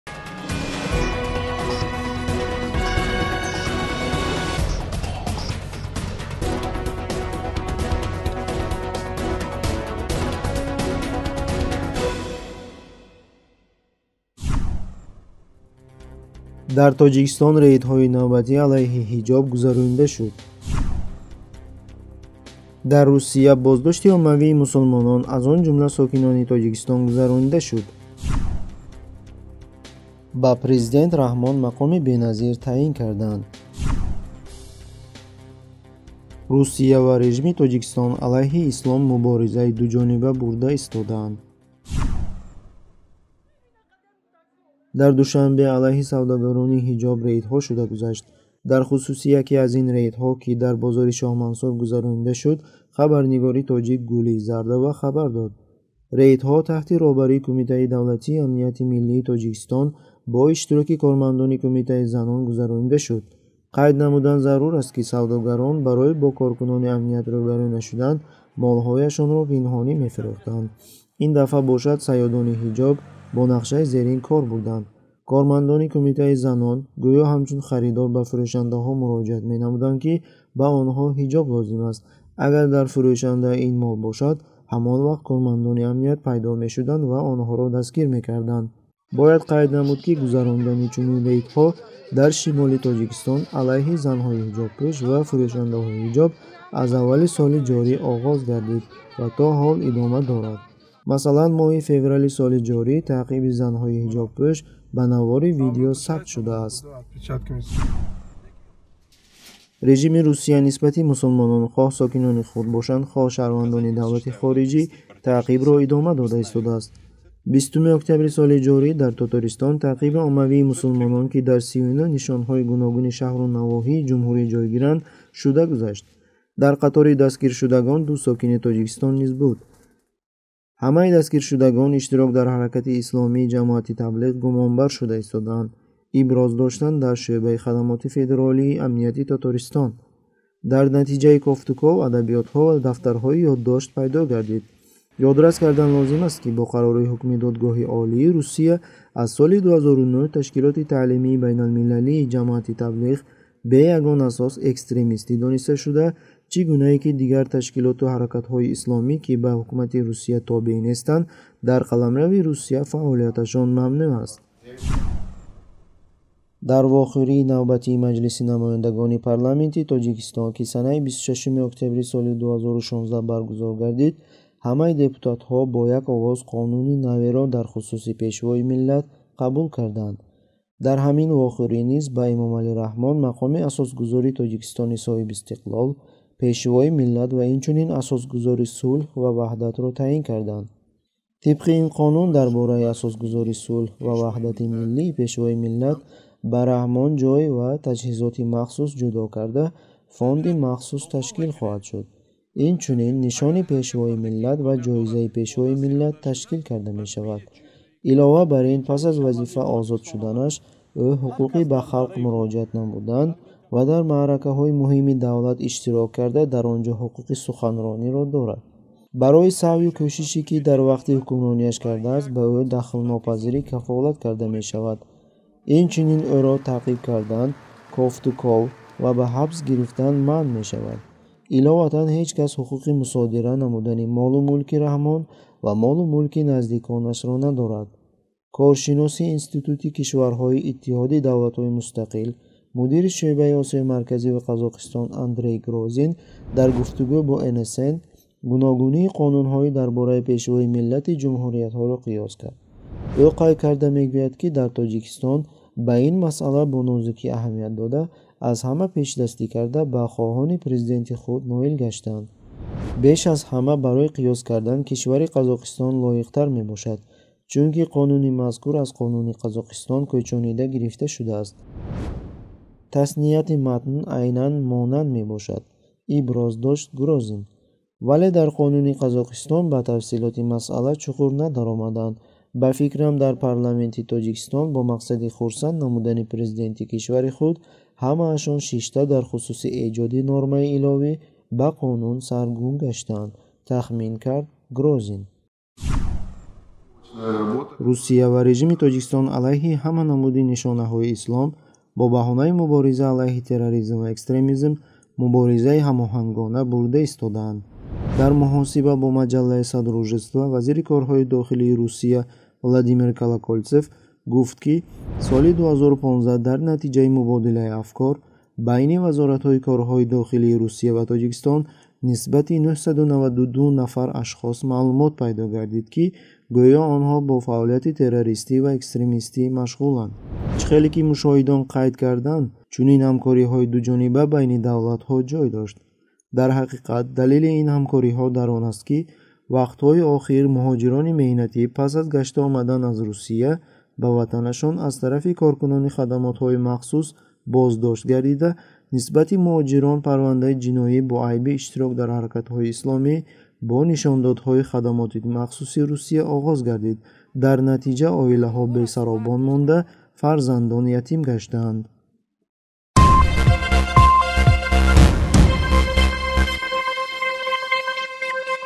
Хабарҳои муҳимтарин аз 06.11.2016
1. Дар Тоҷикистон рейдҳои навбатй алайҳи "ҳиҷоб" гузаронида шуд
2. Дар Русия боздошти оммавии мусалмонон аз он ҷумла сокинони Тоҷикистон гузаронида шуд
3. Ба президент Раҳмон мақоми беназир таъйин карданд
4. Русия ва режими Тоҷикистон алайҳи Ислом муборизаи дуҷониба бурда истодаанд